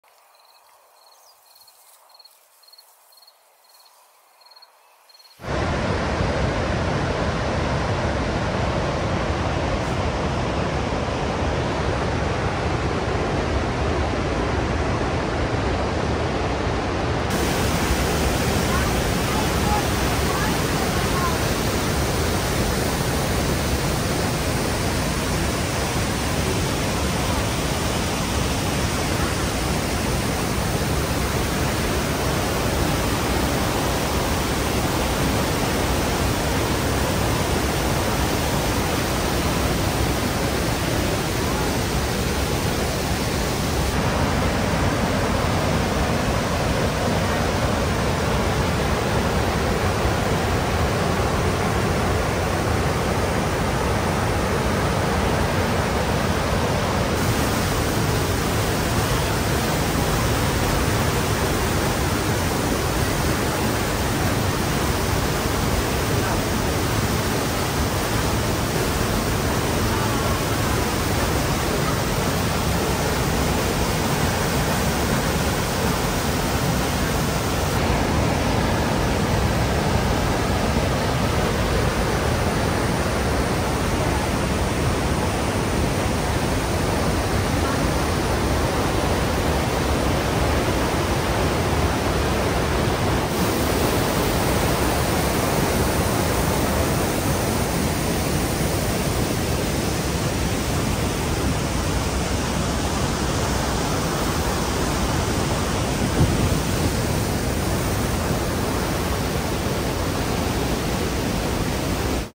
Rhine Falls 4K (Switzerland, 2023) | Amazing Places
The Rhine Falls is a waterfall located in Switzerland and the most powerful waterfall in Europe. The falls are located on the High Rhine on the border between the cantons of Schaffhausen and Zürich